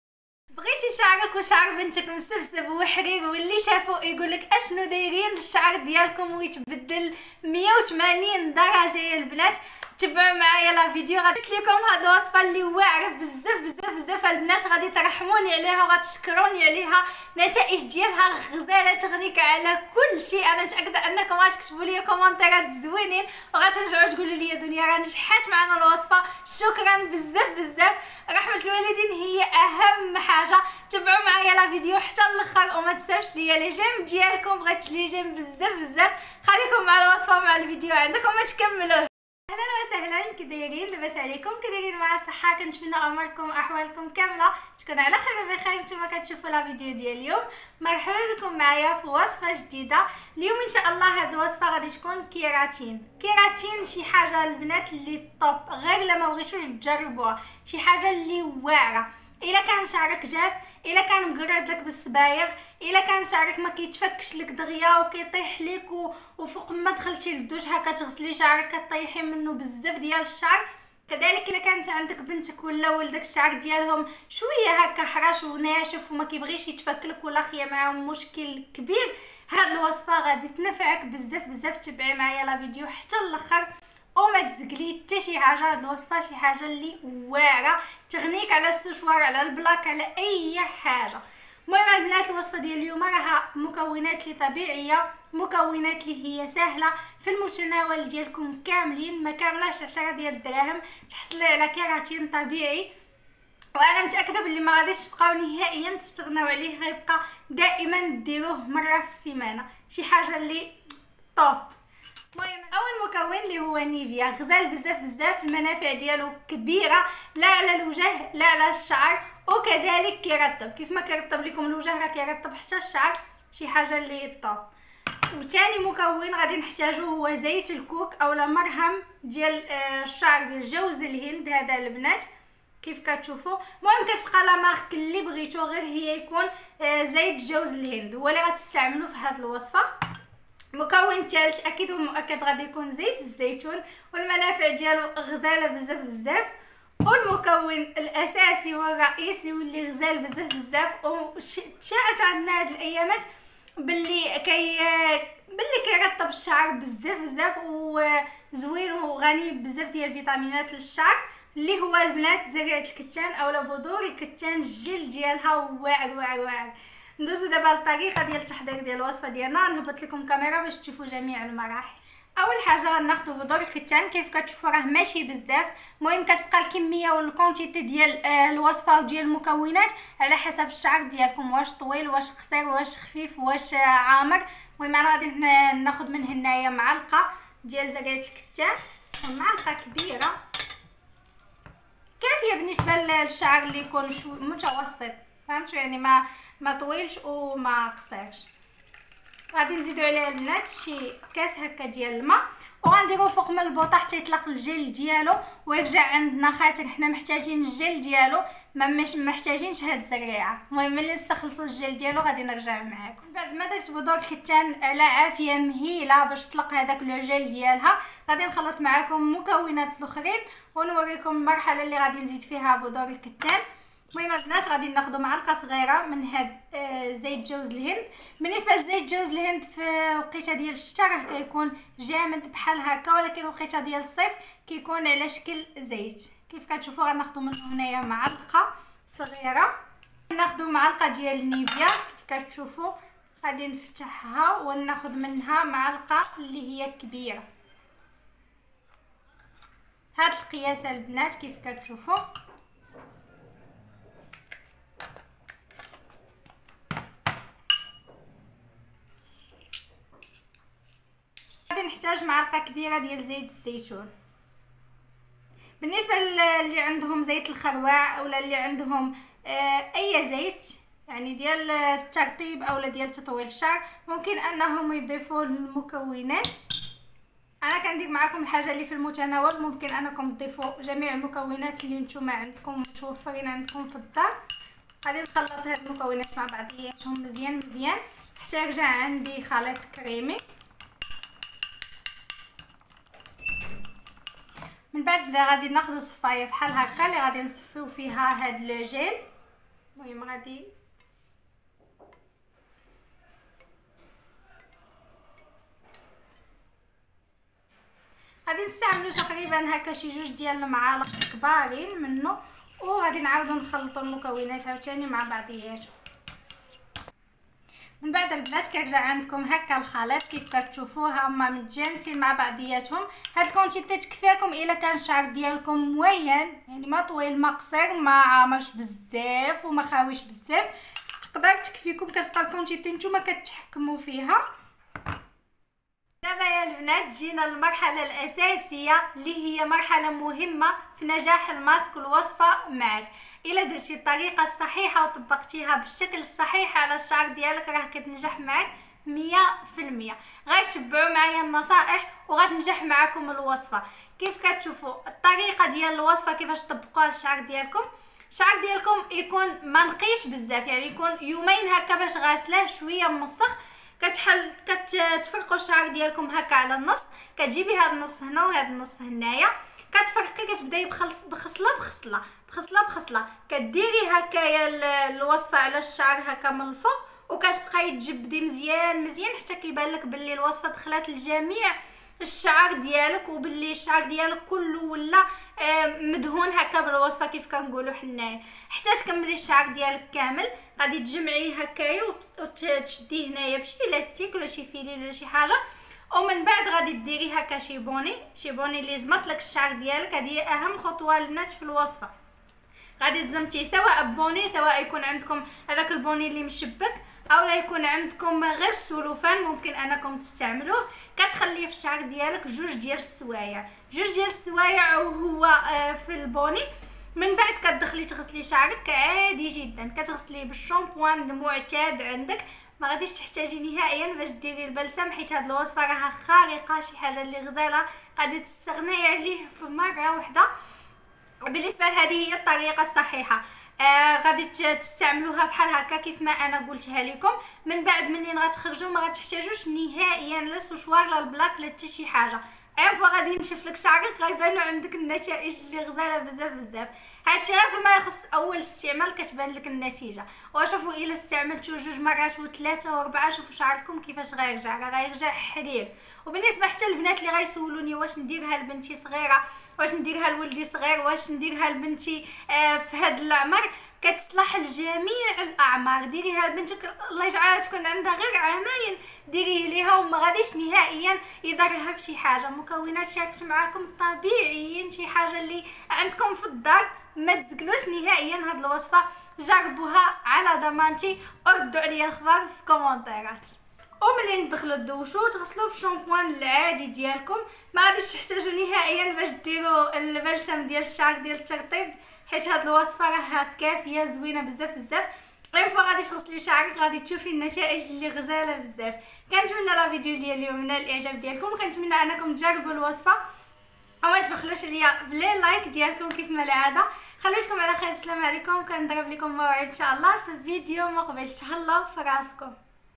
تطويل الشعر بجنون في ليلة واحدة!تطويل الشعر القصير بسرعه الصاروخ وعلاج التساقط بصفة نهائيه
وتفعيل زر الجرس ليصلكم كل جديد ❤️❤️❤️❤️❤️
و أخيرا أقوى كيراتين طبيعي يرطب الشعر الخشن و يطولو